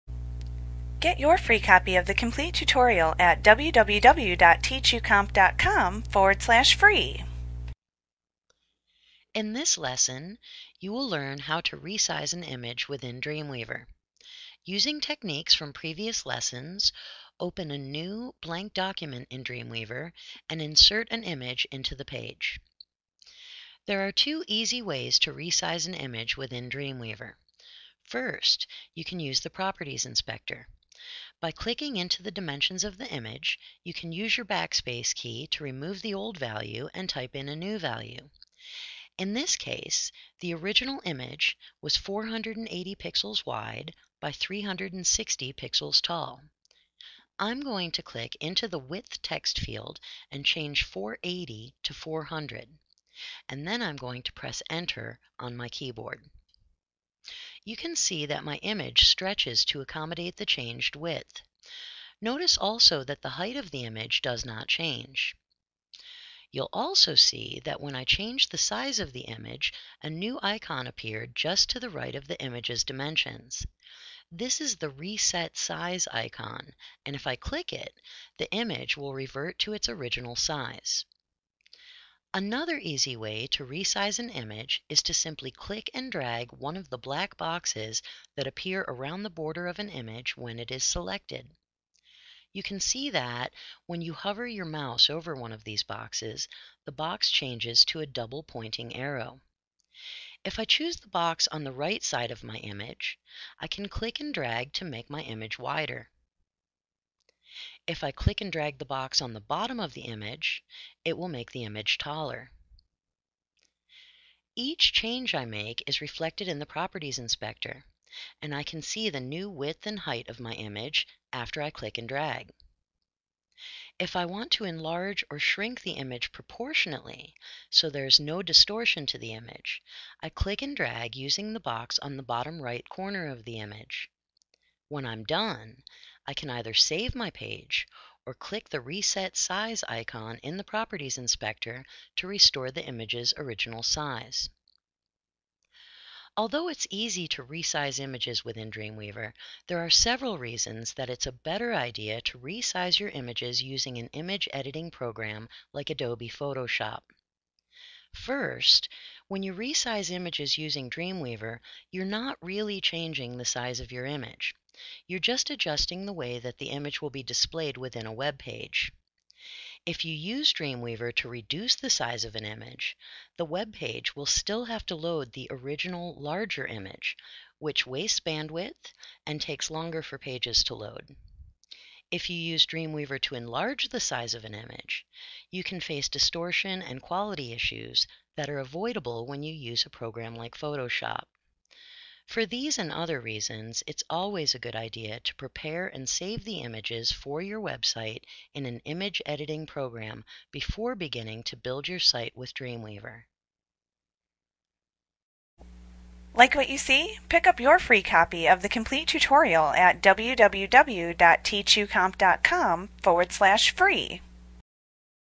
Dreamweaver CS5 Tutorial Resizing Images Adobe Training Lesson 4.8
Learn how to resize images in Adobe Dreamweaver A clip from Mastering Dreamweaver Made Easy v. CS5. - the most comprehensive Dreamweaver tutorial available. Visit us today!